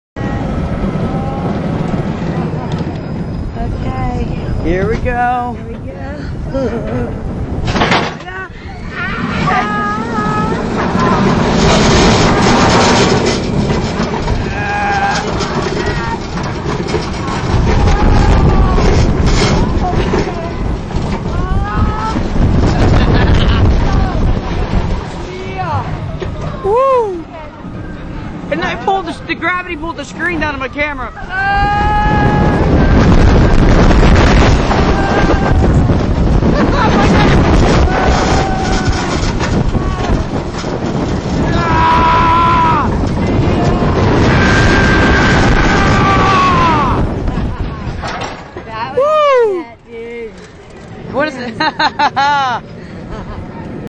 Ridign a rollercoast with my mom @kings island (9-8-2011)
me and my mom in july of 2011